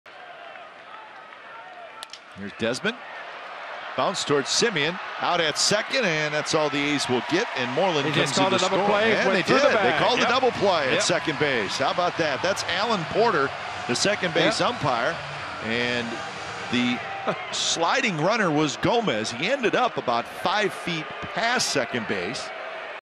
OAK@TEX: Semien starts two after Gomez interferes
Marcus Semien fields Ian Desmond's grounder and turns two after Carlos Gomez interferes with Joey Wendle at second